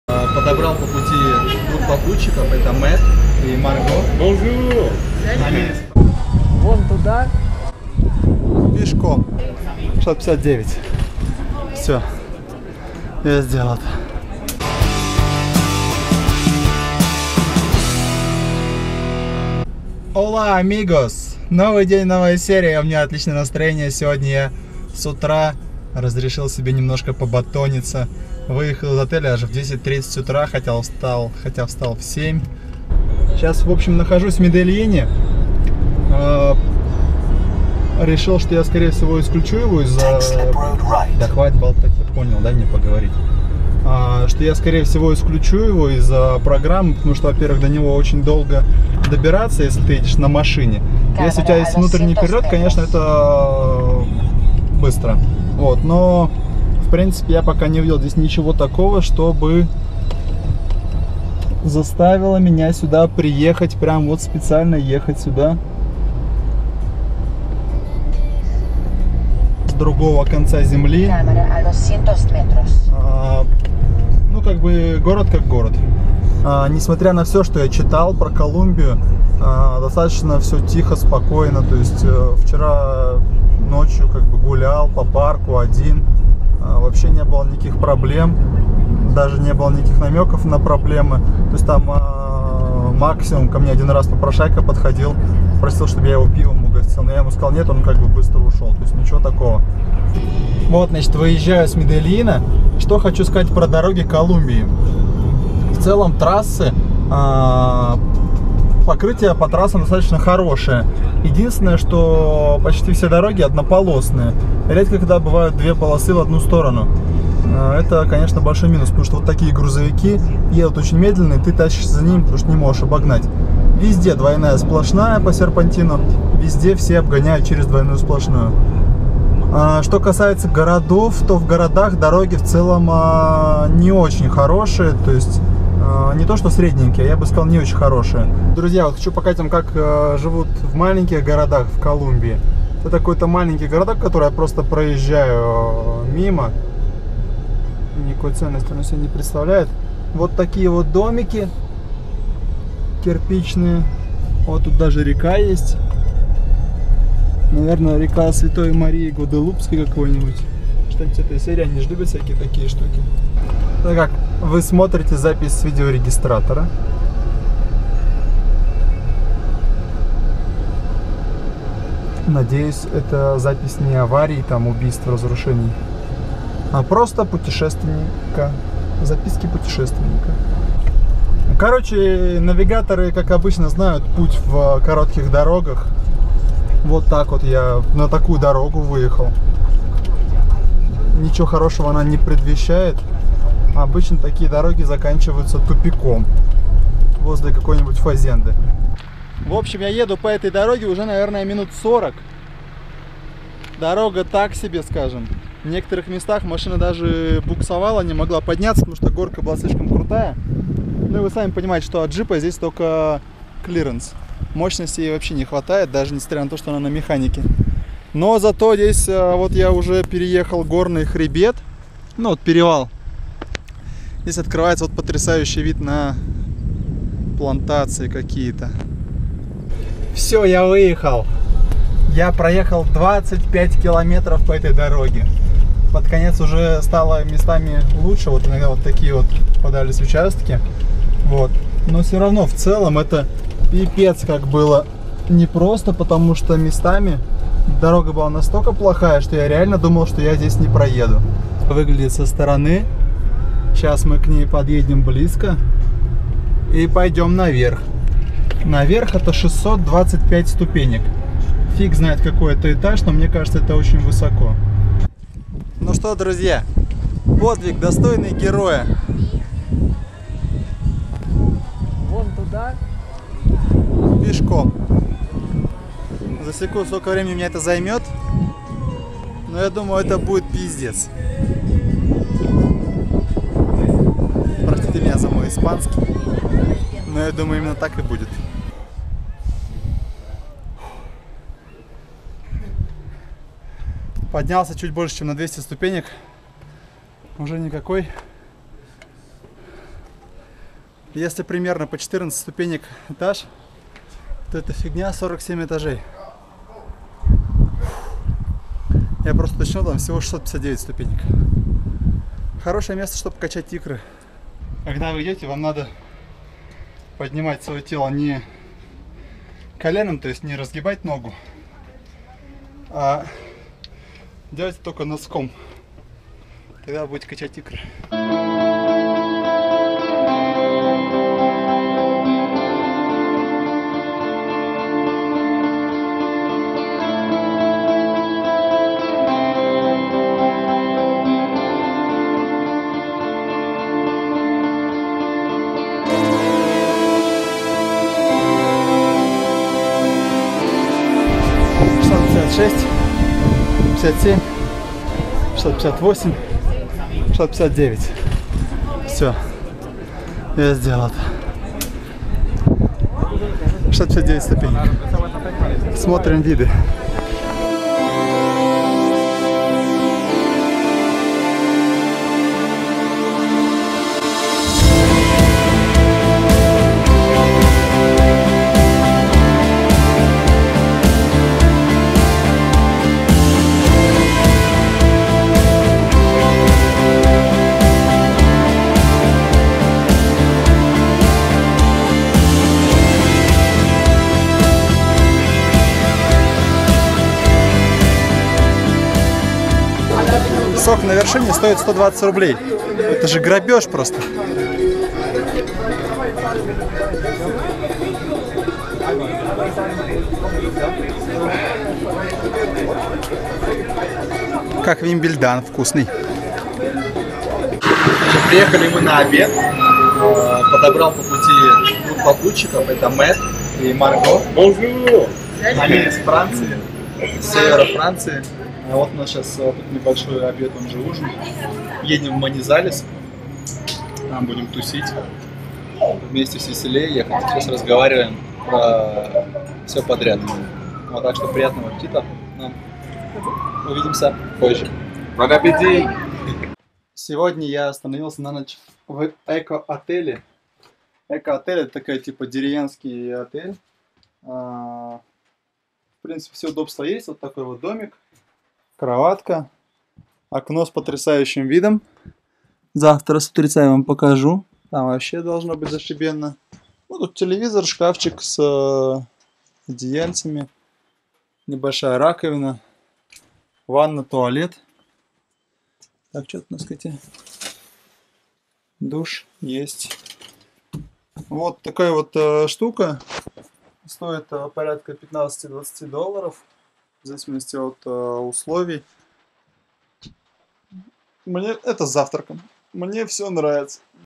Подобрал автостопщиков. Ошибки навигатора. Эко отель.
Весь день ехал на машине, рассказывал про достопримечательности Колумбии и дороги. Подобрал автостопщиков из Франции. Всегда хотелось попробовать путешествовать автостопом, но как то не довелось.
Не люблю ошибки навигатор пытается срезать путь и начинает тебя вести непонятными дорогами. В общем мой навигатор завел меня в дикие края, куда-то в горы Колумбии, где местные фермеры выращивают всевозможные легальные и не очень растения)
Приехал в городок Гуадапе, чтобы подняться на огромную скалу пешком. Прошел 659 ступенек, зато вы узнаете как накачать икры на леснице. Остановился в маленькой деревне в горах Колумбии в эко отеле. Чаще всего это частные дома, которые люди сдают в аренду. Минимум удобств, максумум выгоды и аутентичности!